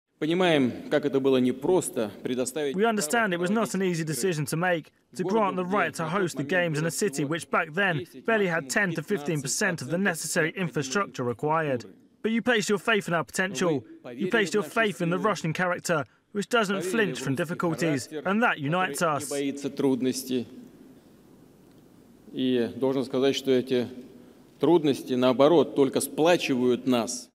Putin pleased faith was place in Russia to host Winter Olympics
International Olympic Committee (IOC) President Thomas Bach criticised world leaders for politicising the Olympic Games as Russian President Vladimir Putin welcomed delegates to Sochi on Tuesday (4 February).